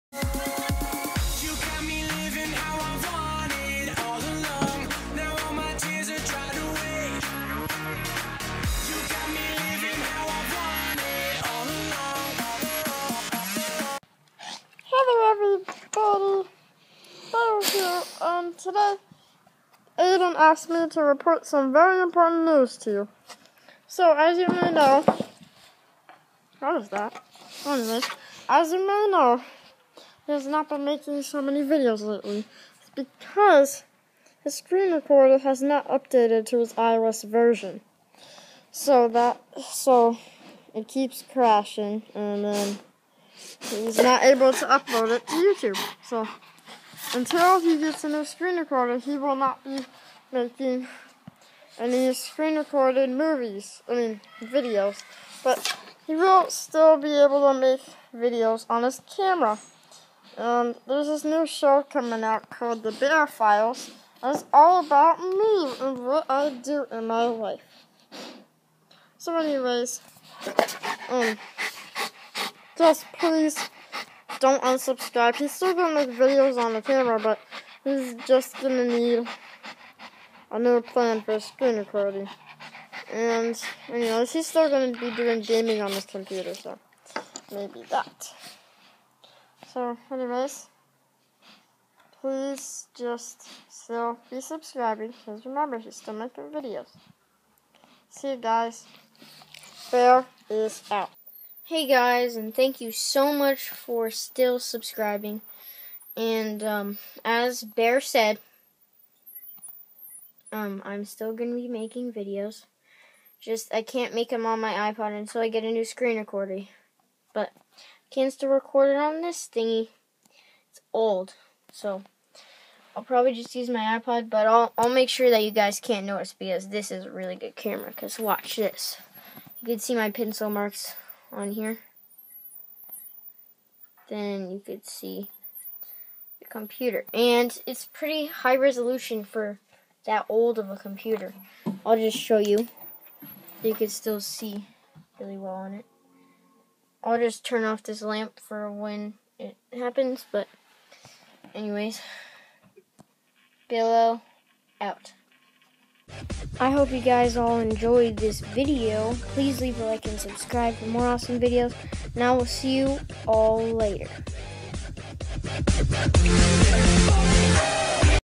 UPDATE VLOG with Bear!MOVIES,SHOWS!
Making movies now and don't worry guys, I am still making animations. Some are coming up!